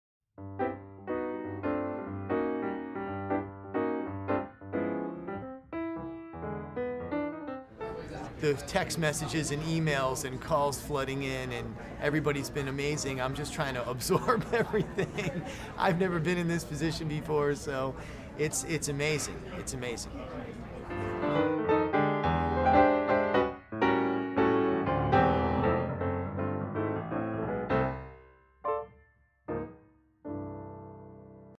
Talkin Tonys - John Clancy (Shrek The Musical)
John Clancy, 2009 Tony Award nominee for Best Orchestrations (Shrek The Musical), talks about how it feels to be nominated for a Tony Award.